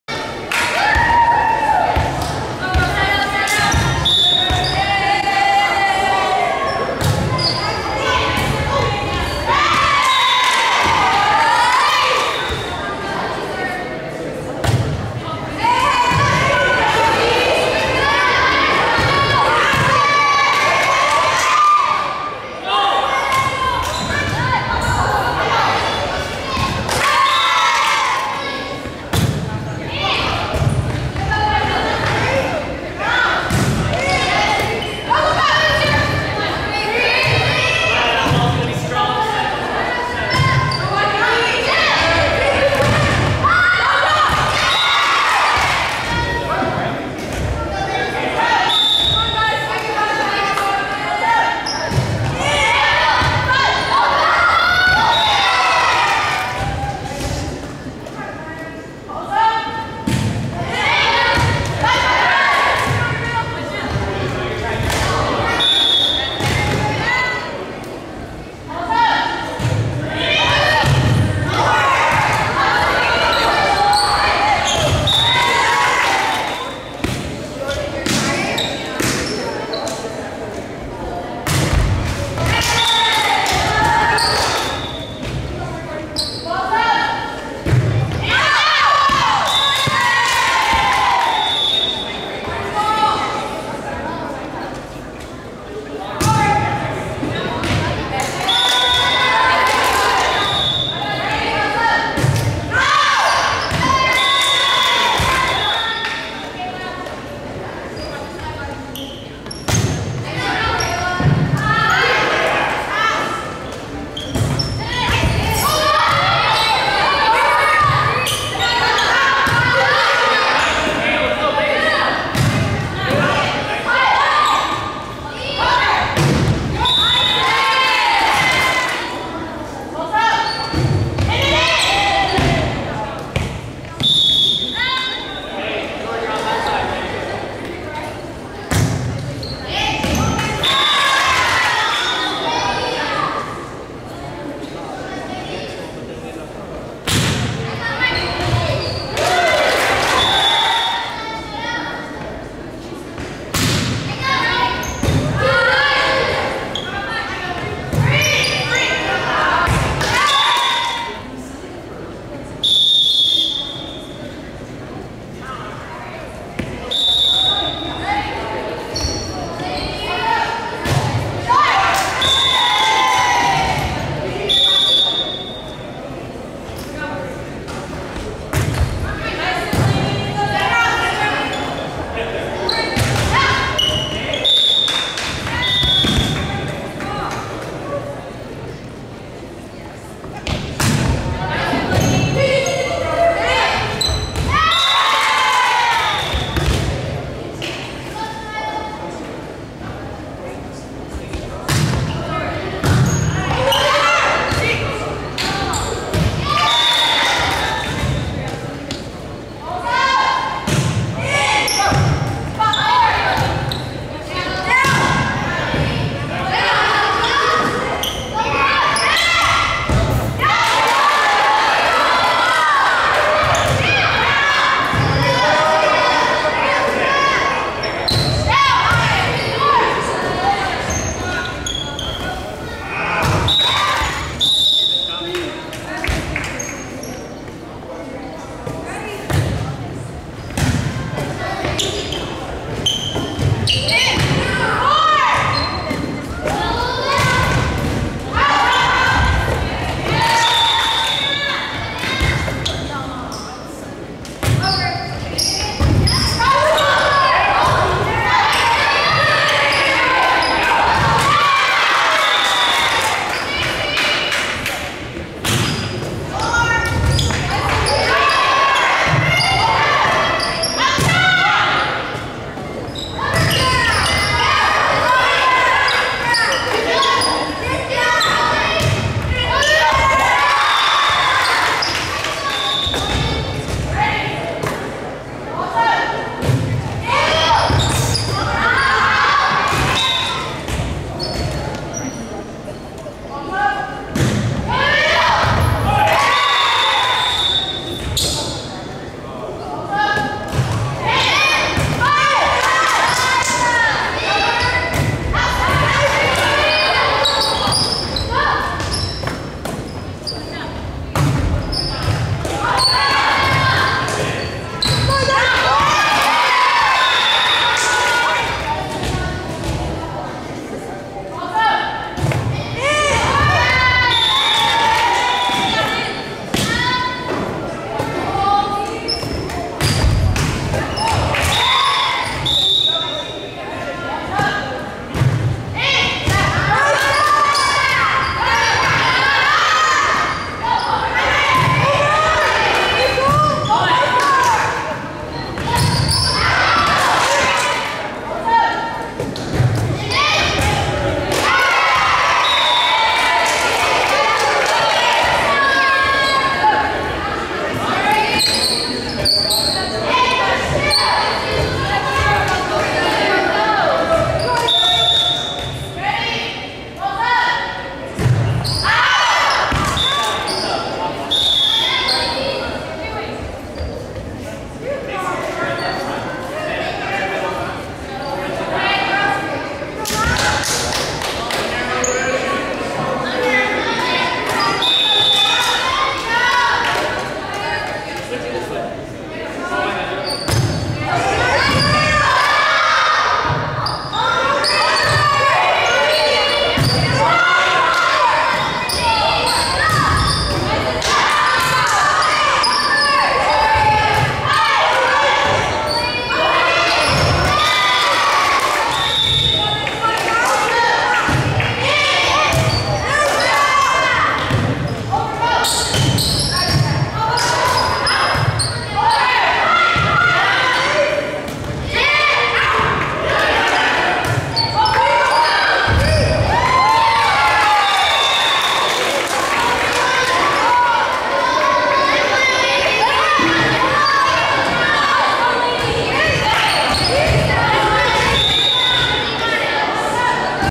2019 GVB Tenaya (Bullard) at Clark (Clovis) Girls volleyball GM 2
Girls Middle School volleyball Tenaya (Bullard feeder) of Fresno vs Clark (Clovis feeder)